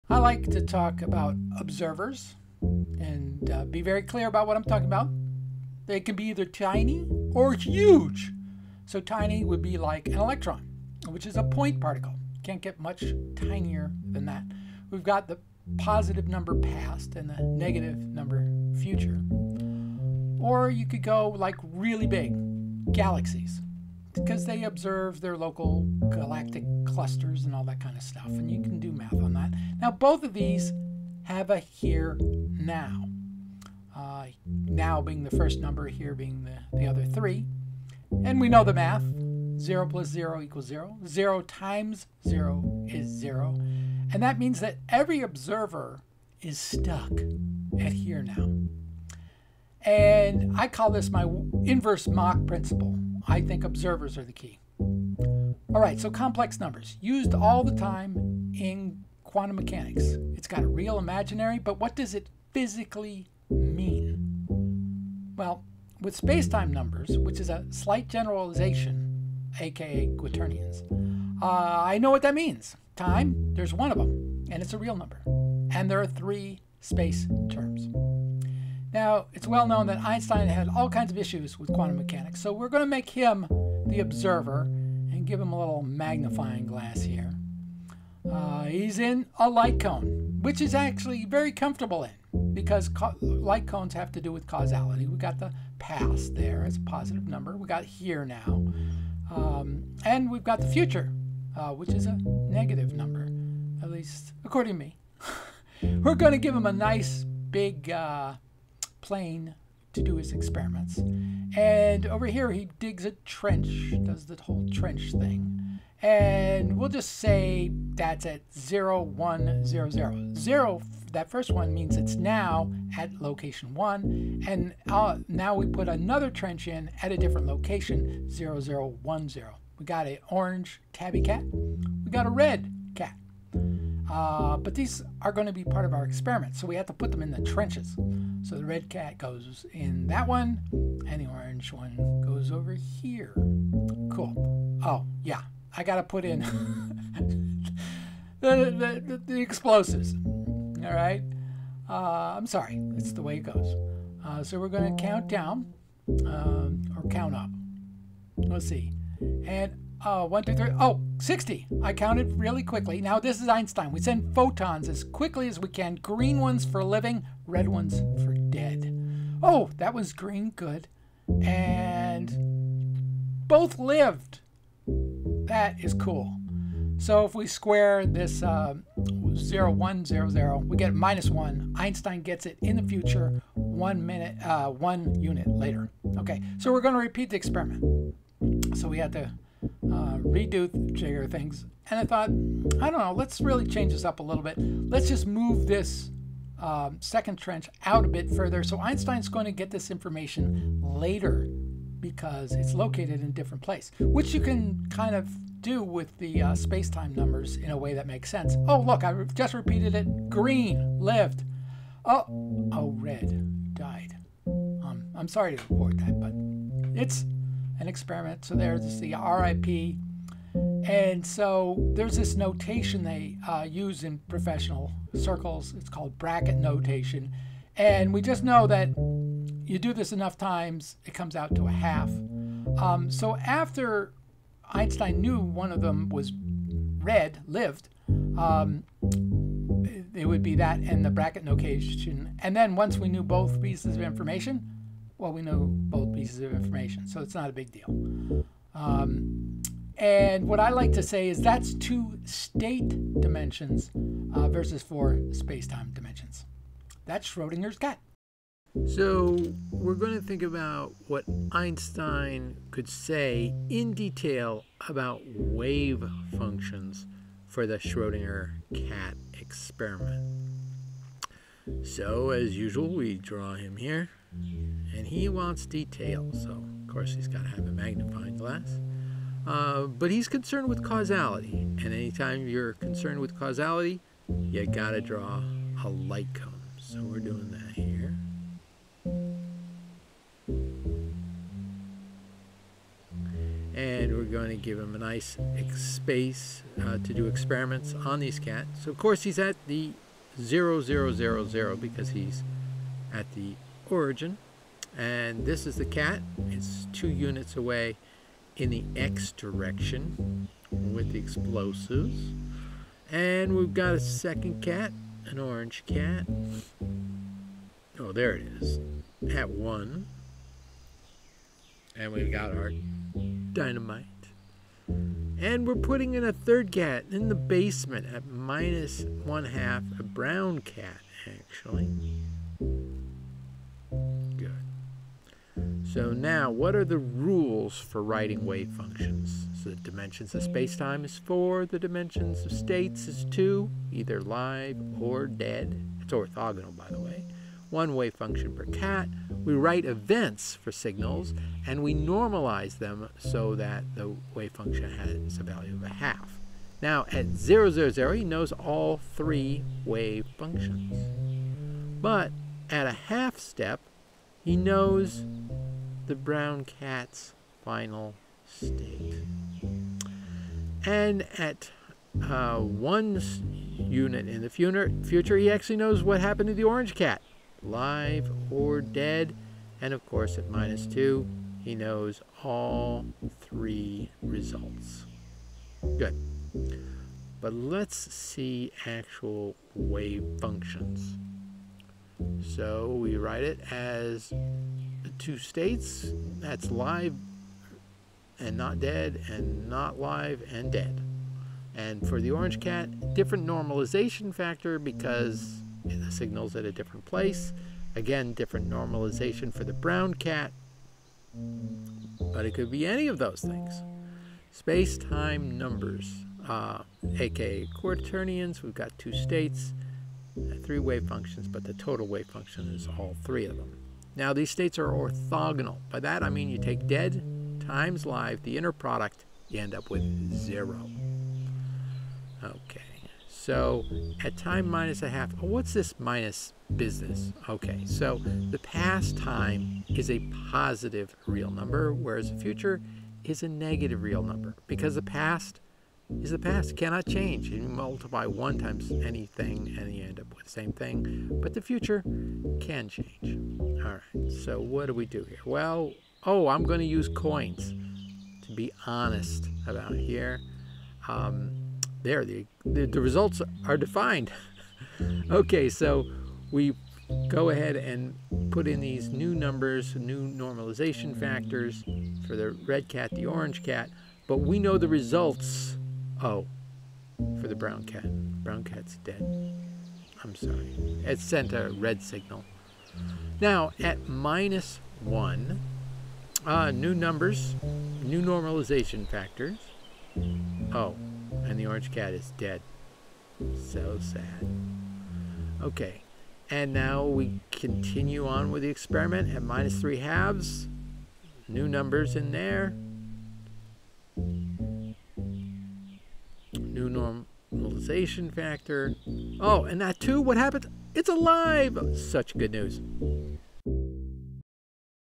Understanding Schrödingers cats, Parts 1 & 2, Space-like Space-time Numbers and The Wave Function
In 2011, Henry Reich posted a great video on the Schrödinger's cat experiment, available This is my effort to explain the thought experiment. Space-time numbers (aka quaternions) are used instead of complex numbers because space-time numbers live at obvious places in a space-time diagram. Einstein gets to use a light cone which is needed to understand causality. The events that are in the wave function are space-like separated from the observer (Einstein in this case).

In Part 1, space-time numbers are introduced as a way to explain the Schrödinger's cat thought experiment. At the start of the experiment where each individual cat is alive or dead, a signal is sent from the bunker to Einstein who is the observer. Einstein knows when he will receive the signal. Until then, the best he can describe the results of the experiment is with the wave function.

In Part 2, the wave function is drawn at different moments in space-time: the start, then each half step afterward as Einstein eventually learns the fate of each of the three cats. The video was tried a few times. As a random process was used, one time all the cats lived, another they all died. You will have to watch to see what got uploaded here.